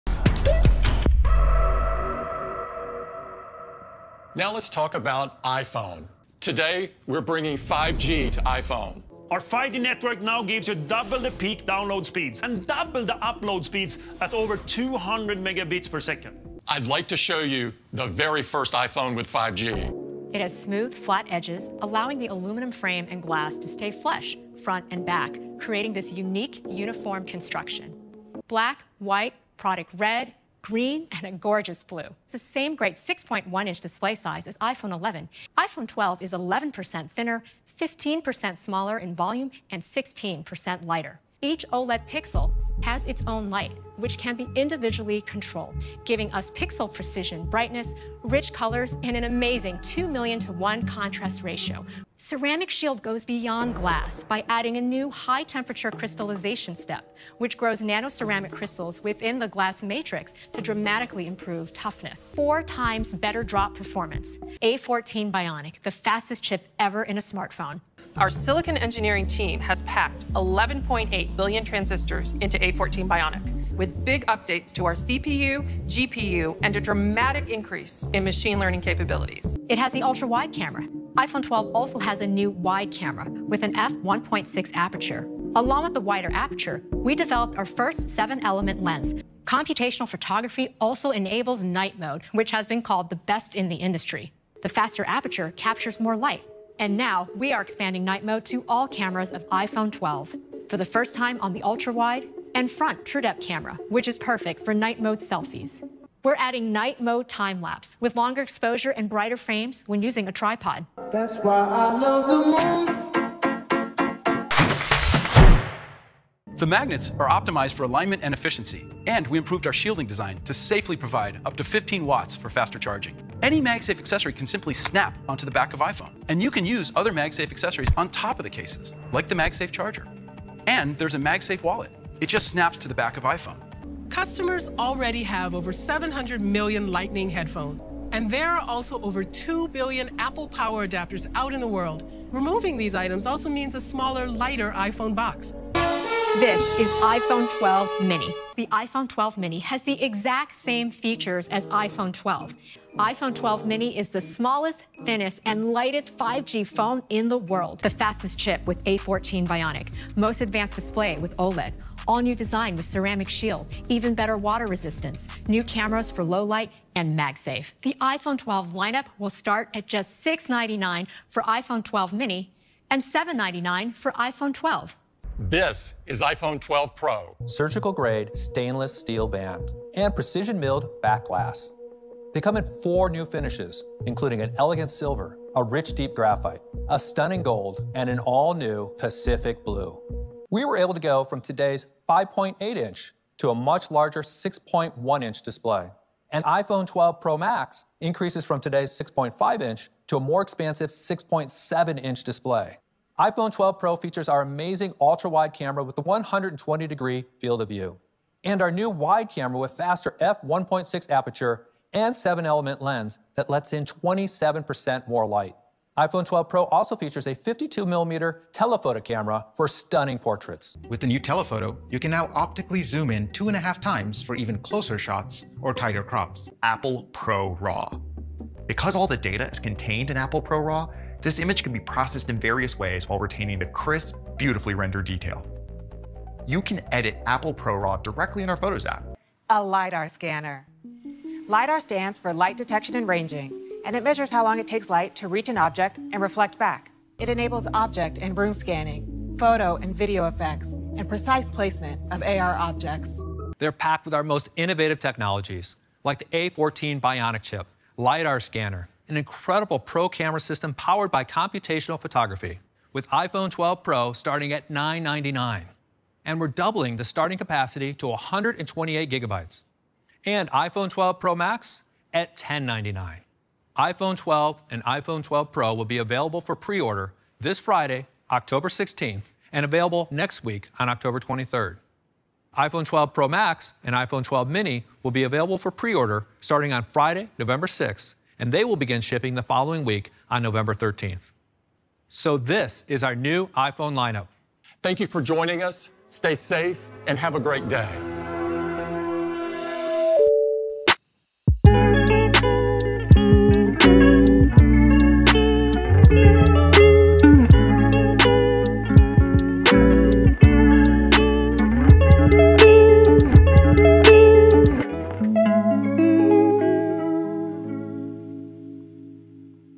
Apple's iPhone 12 Event in 5 Minutes | Mashable Highlights
Apple's new iPhones have finally been revealed! Catch up on all the details you need to know in just 5 minutes.